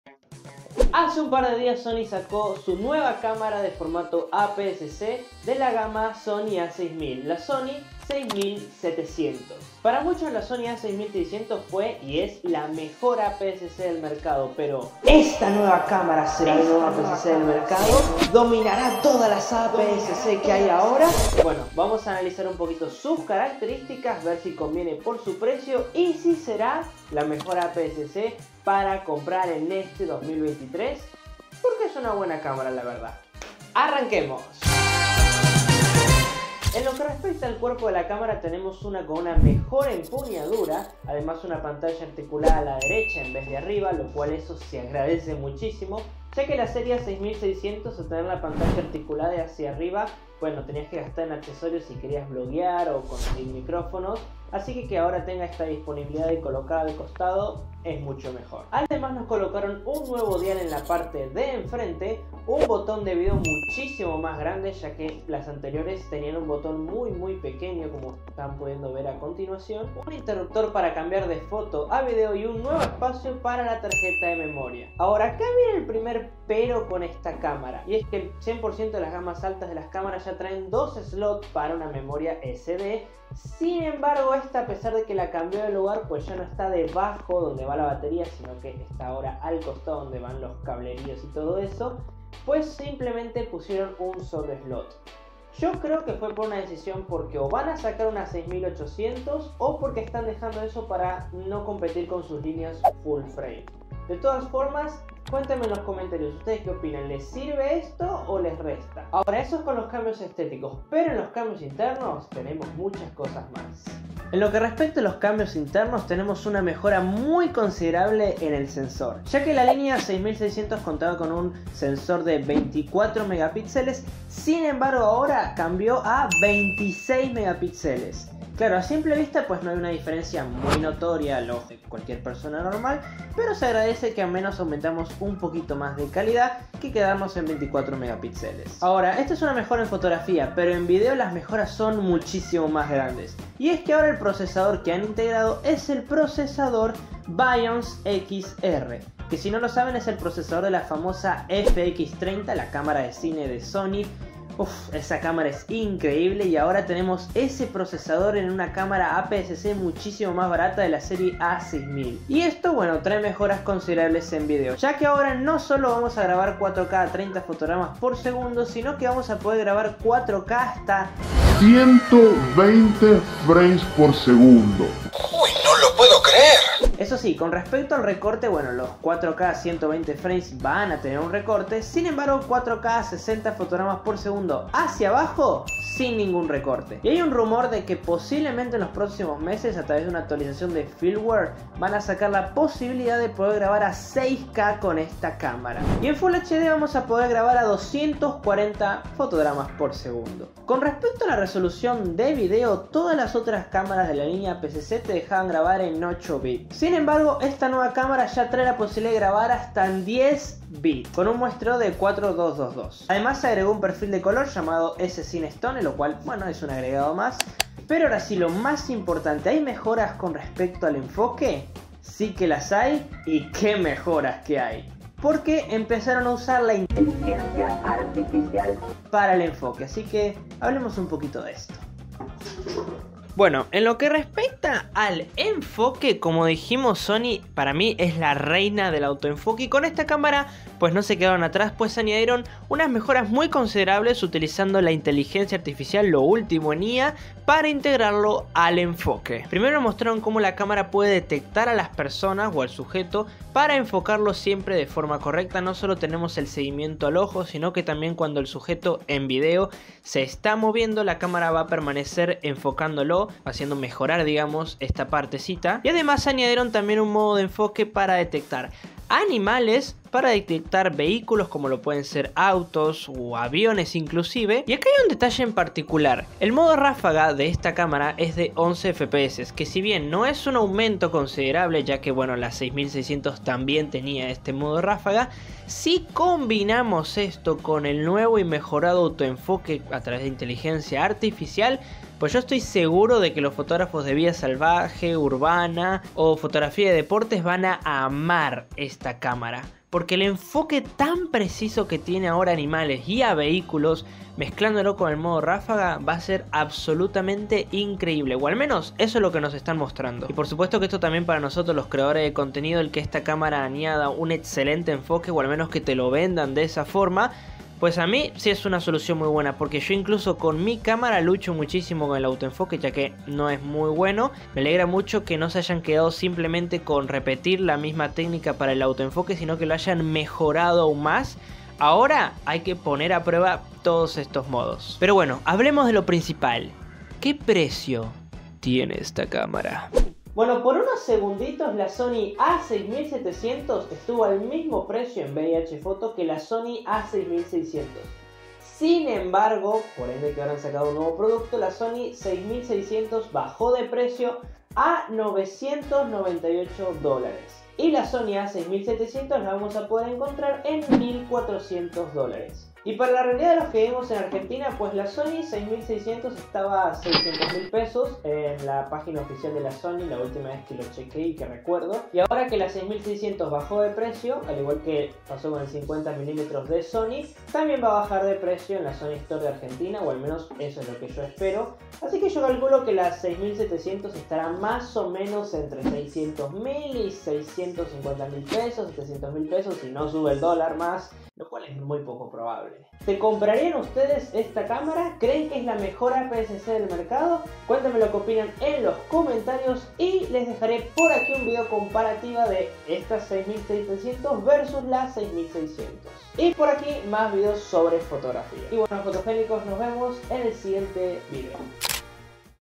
LA MEJOR CÁMARA APSC que puedes COMPRAR
En este video les vamos a analizar la nueva cámara de SONY la SONY A6700, ¿será la MEJOR cámara APS-C del mercado? averigüémoslo!!
Esta cámara tiene un sensor de 26 megapíxeles, cuenta con un sistema de autoenfoque con inteligencia artificial con varios seguimientos, tanto a personas, animales y objetos, una sistema de ráfaga de 11 fotografías por segundo. Además trae mejoras en video capaz graba en 4k a 60fps sin recorte y en 4k hasta 120 fps sin recorte, grabación de video a 10bit.

Mi equipo:

🎥Cámaras:

-Canon T5i=

📸Lentes:

-18-55 F3.5=

-50mm f1.8

🎤Audio:

-Micrófono Synco SY-S6E-LAV=

🔭Trípode:

-Tripode SIRUI=

🔔Memoria:

-Tarjetas SD=